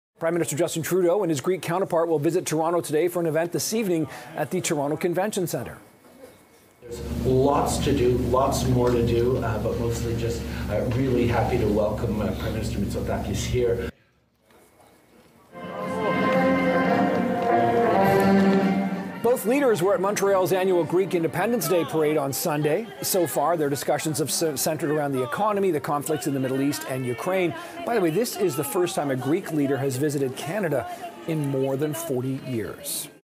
Justin Trudeau and Greek counterpart in Toronto today
Prime Minister Justin Trudeau and his Greek counterpart will visit Toronto today for an event this evening at the Toronto Convention Center.